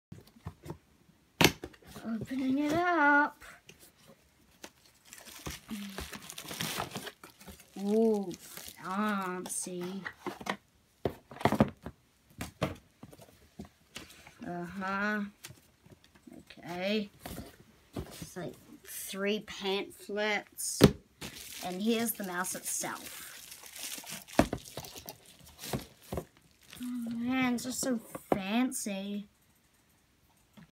Unbox HP gaming mouse
Quick video of me unboxing my new HP gaming mouse.